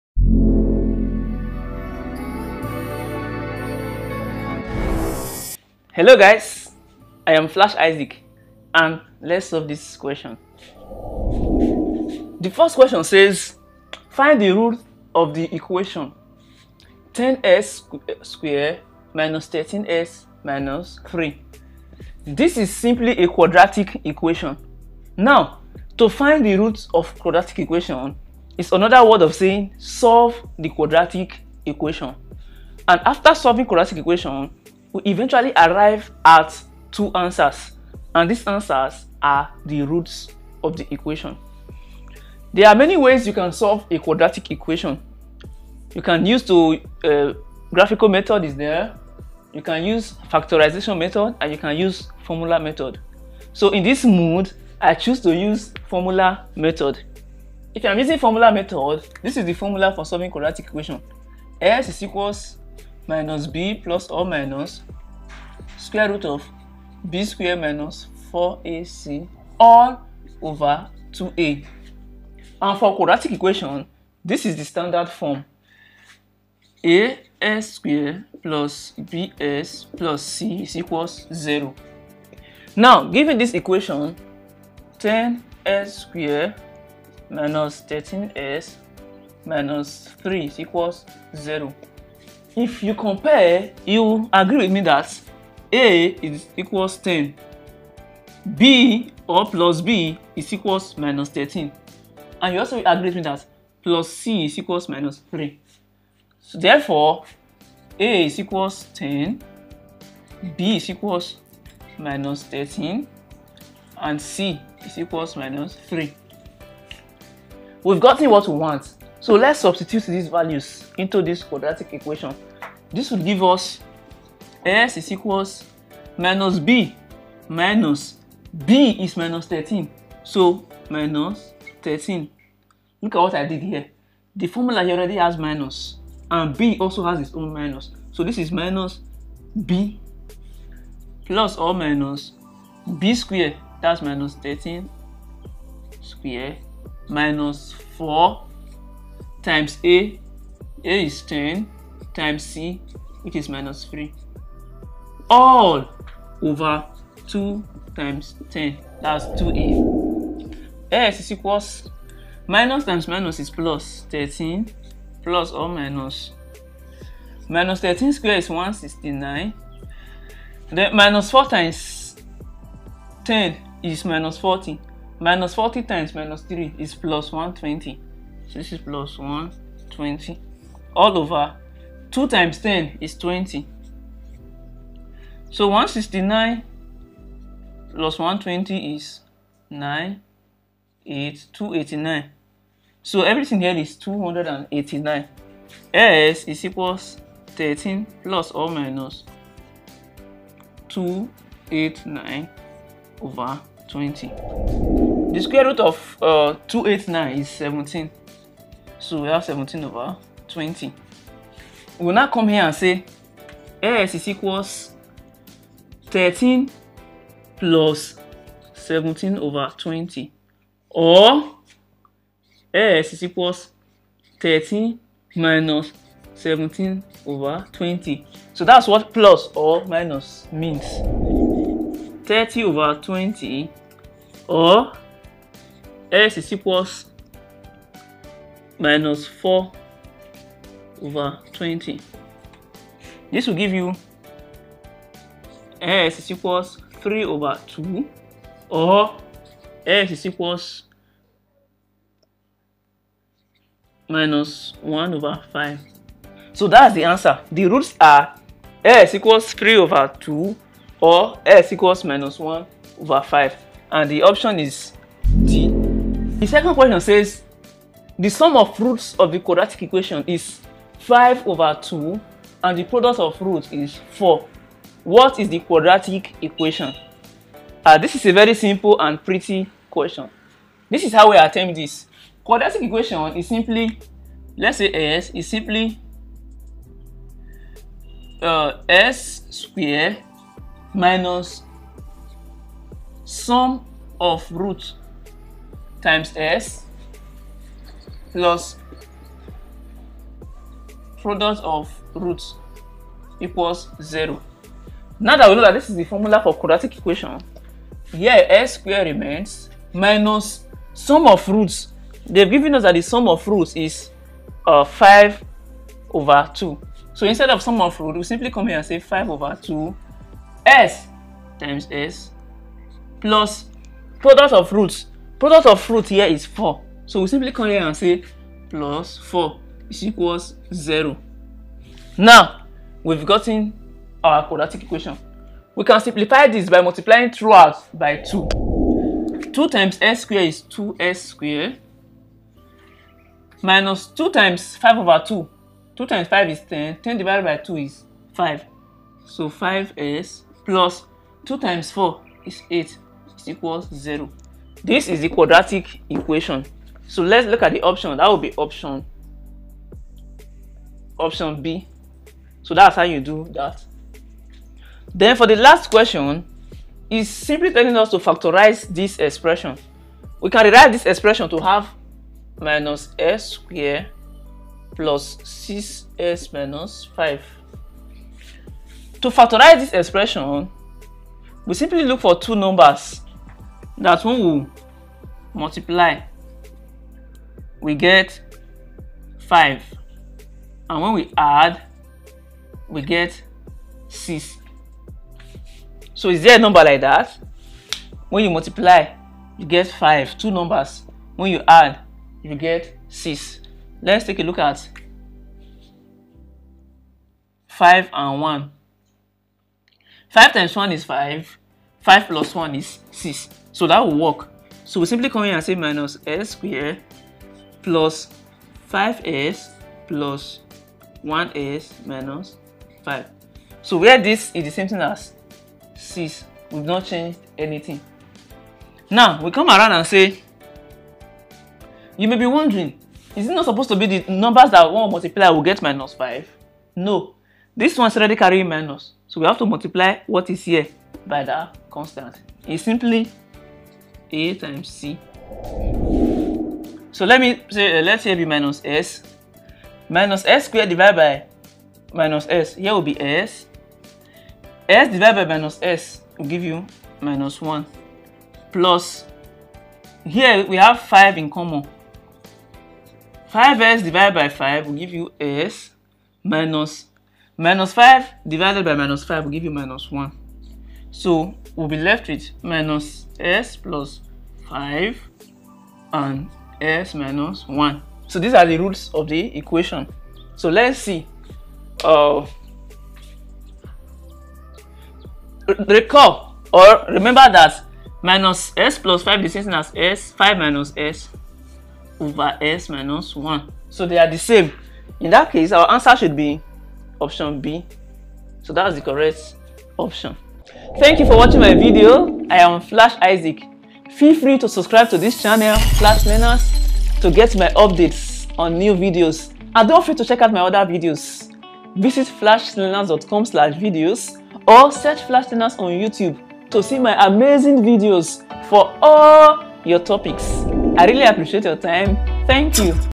UI Post UTME | Quadratic Equation Questions
University of Ibadan Mathematics Post UTME Questions and Answers Under Quadratic Equation. These are the frequently set questions in University of Ibadan (UI) Post UTME Screening.